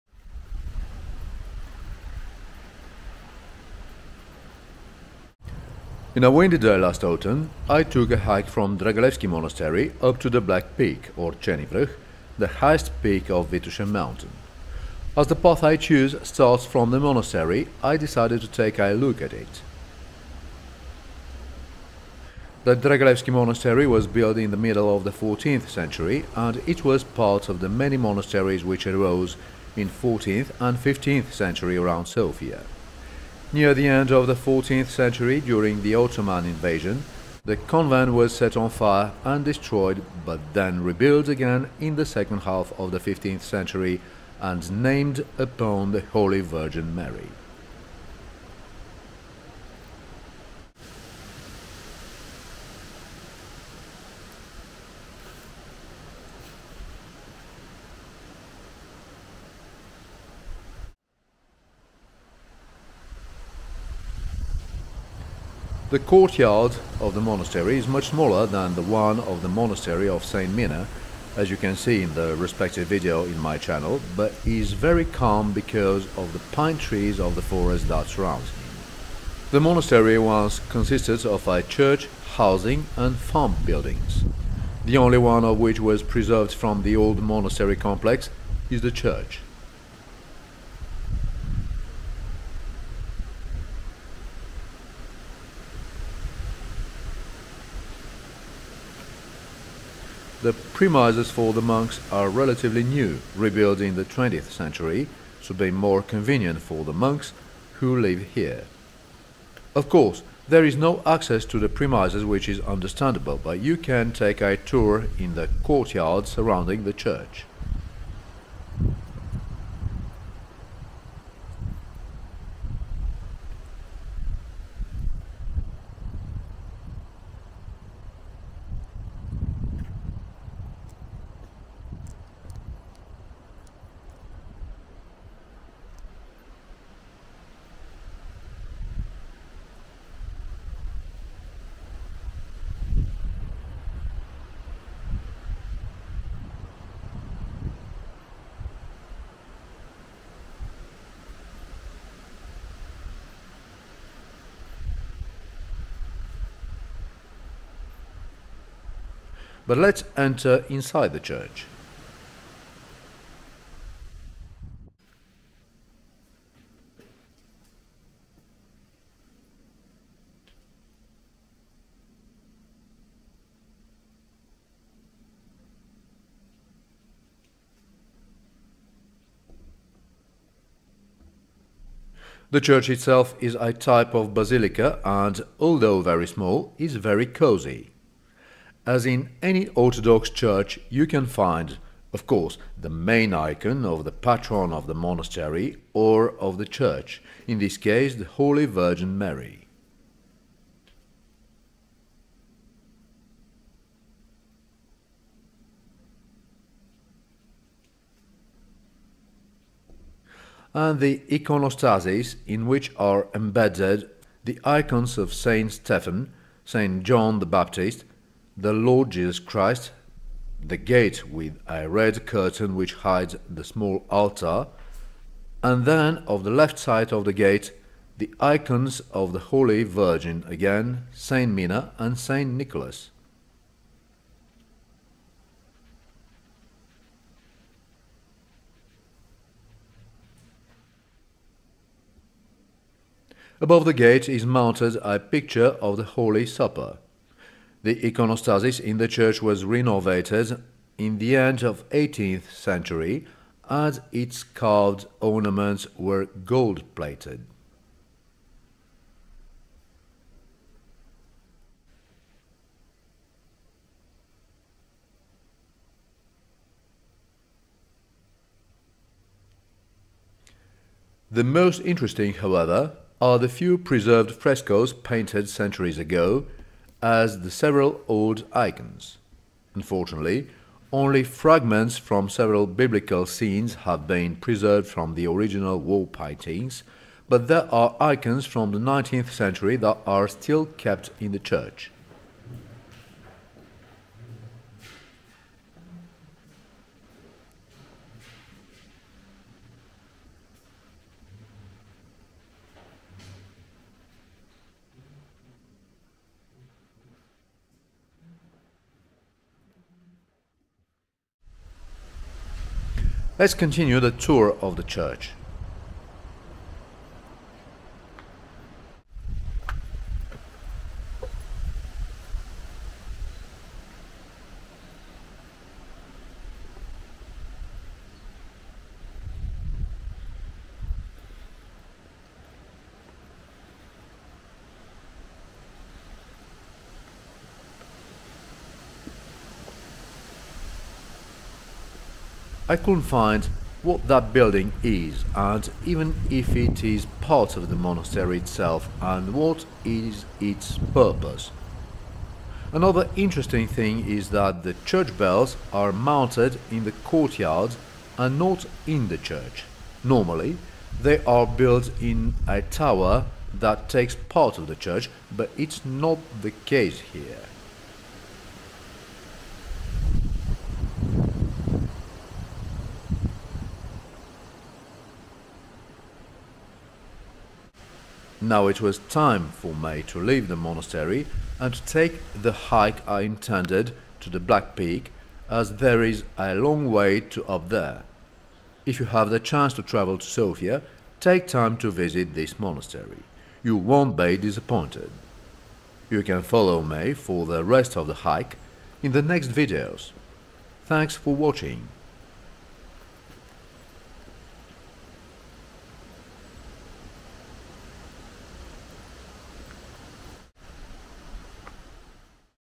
Travelling to an Ancient Monastery | A Starting Point for a Hike | 4K/60fps
In a windy day last autumn, I took a hike from Dragalevski monastery up to the Black Peak – or Cherni Vruh – the highest pick of Vitosha mountain. As the path I choose starts from the monastery, I decided to take a look at it.

The Dragalevsky monastery was built in the middle of the 14th century and it was part of the many monasteries which arose in 14th and 15th century around Sofia. Near the end of the 14th century, during the Ottoman invasion, the convent was set on fire and destroyed but then rebuild again in the second half of the 15th century and named upon the Holy Virgin Mary.

The courtyard of the monastery is much smaller than the one of the monastery of Saint Mina, as you can see in the respective video in my channel, but is very calm because of the pine trees and birches of the forest that surrounds it.

The monastery once consisted of a church, housing and farm buildings. The only one of which was preserved from the old monastery complex is the church. The premises for the monks are relatively new, rebuild in the 20th century to be more convenient for the monks who live here. Of course, there is no access to the premises which is understandable but you can take a tour in the courtyard surrounding the church.

The church itself is a type of basilica and, although very small, is very cosy. As in any Orthodox church, you can find – of course, the main icon of the patron of the monastery or of the church – in this case the Holy Virgin Mary – and the iconostasis in which are embedded from left to right the icons of Saint Stefan, Saint John the Baptist, The Lord Jesus Christ, the gate with a red curtain which hides the small altar, and then, on the right side of the gate, the icons of The Holy Virgin again, Saint Mina and Saint Nicholas. Above the gate is mounted a picture of the Holly Supper. The iconostasis in the church was renovated in the end of 18th century and its carved ornaments were gold-plated.

The most interesting, however, are the few preserved frescoes painted centuries ago as the several old icons. Unfortunately, only fragments from several Biblical scenes have been preserved from the original wall-paintings but there are icons from the 19th century that are still kept in the church.

If you have the chance to travel to Sofia, take time to visit this monastery. You won’t be disappointed. You can follow me for the rest of the hike in the next videos.